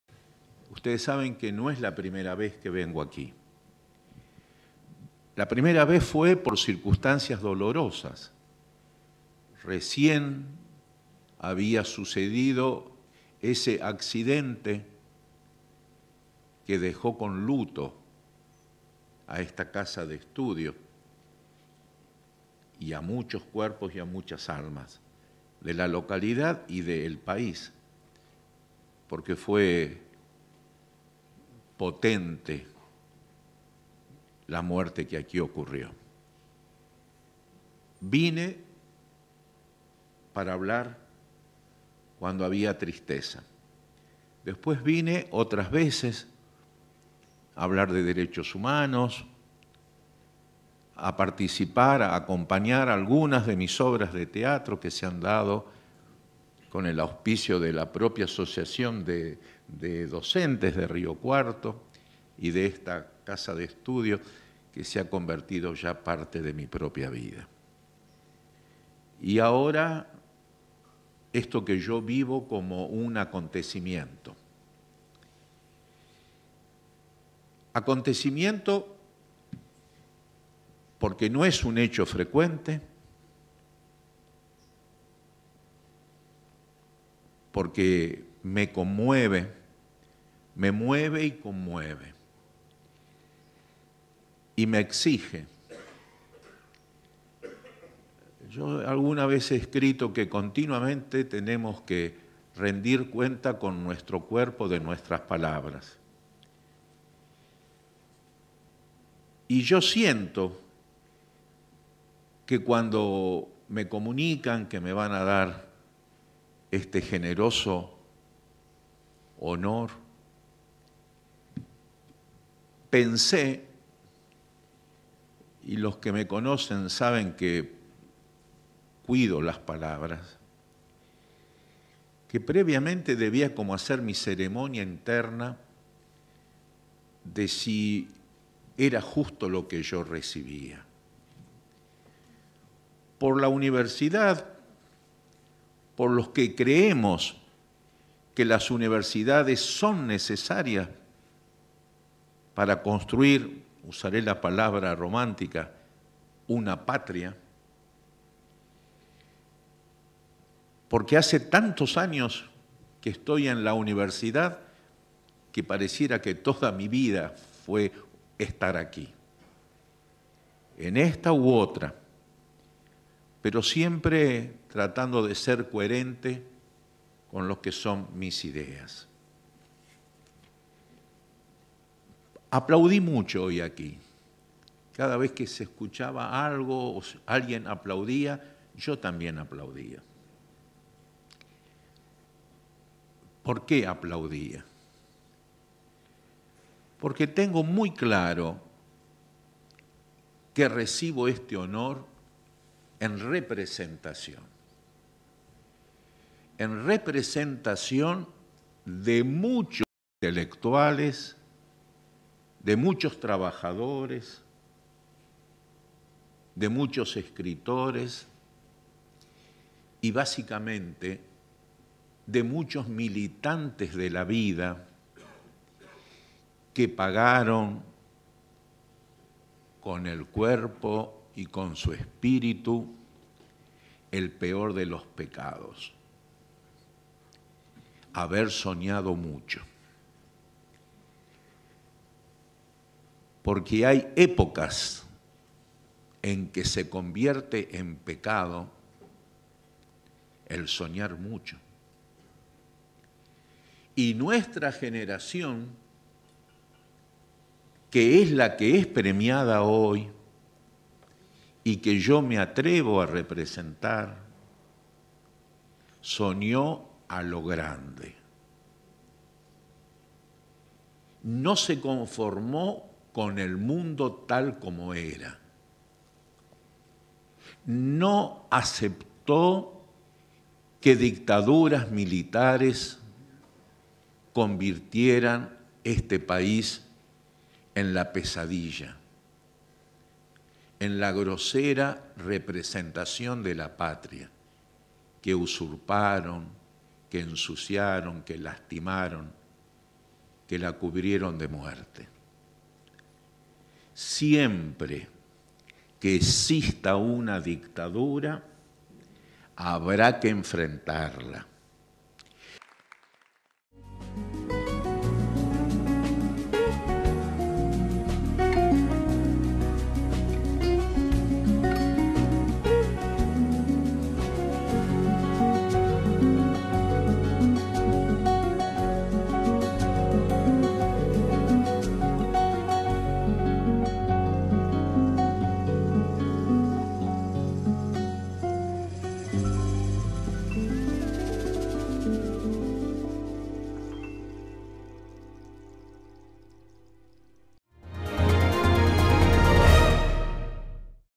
FALLECIÓ VICENTE ZITO LEMA: ESCRITOR, ABOGADO E HISTÓRICO MILITANTE DE DERECHOS HUMANOS
EN 2013 FUE DISTINGUIDO COMO DOCTOR HONORIS CAUSA DE LA UNRC